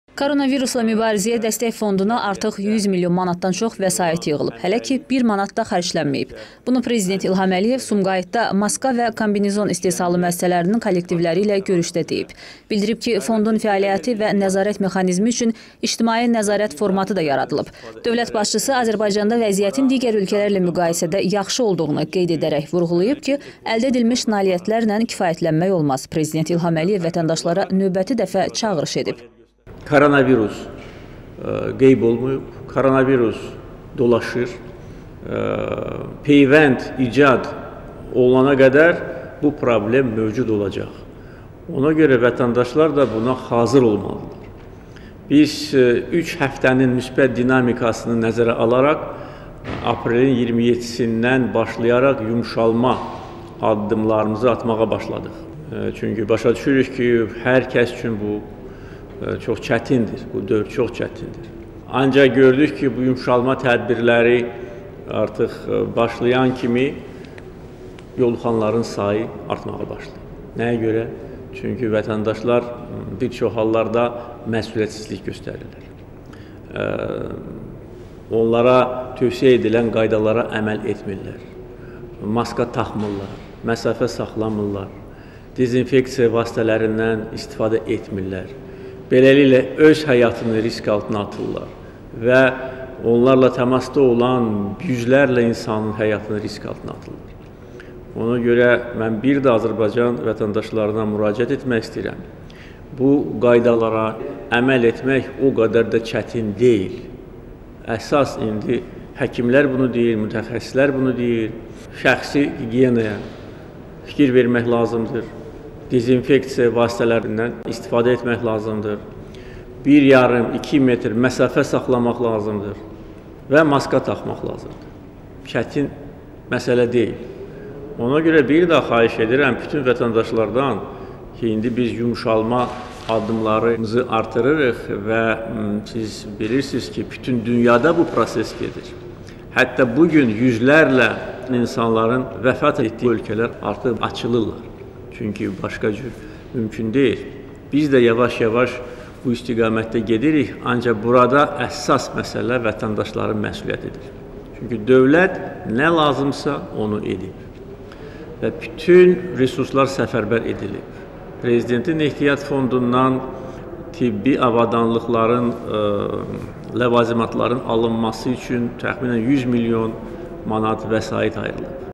DÖVLƏT BAŞÇISI VƏTƏNDAŞLARA ÇAĞIRIŞ EDİB
Koronavirusla Mübarizəyə Dəstək Fondunda artıq 100 milyon manatdan çox vəsait yığılıb. Hələ ki, 1 manat da xərclənməyib. Bunu prezident İlham Əliyev Sumqayıtda maska və kombinezon istehsalı müəssisələrinin kollektivləri ilə görüşdə belə deyib. Bildirib ki, Fondun fəaliyyəti və nəzarət mexanizmi üçün ictimai nəzarət formatı da yarılıb. Dövlət başçısı Azərbaycanda vəziyyətin digər ölkələrlə müqayisədə yaxşı olduğunu deyərək qeyd edib ki, amma əldə edilmiş nailiyyətlərlə kifayətlənmək olmaz. Prezident İlham Əliyev vətəndaşlara növbəti dəfə çağırış edib.